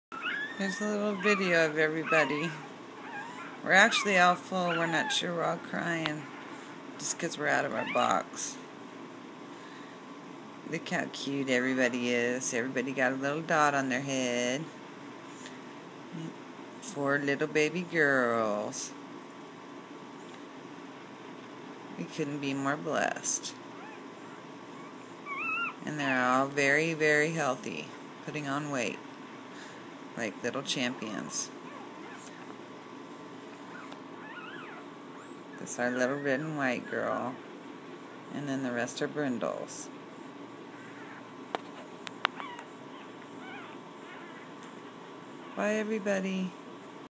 English Bulldog puppies for sale-- Little River English Bulldogs
2 days old.  All girls. Sire and Dam are our family pets. Exceptional champion pedigrees. $1500 each. Includes $200 deposit. Shipping not included. We are located in Wichita, Ks.  We have a wonderful shipping company that we use if you are long distance..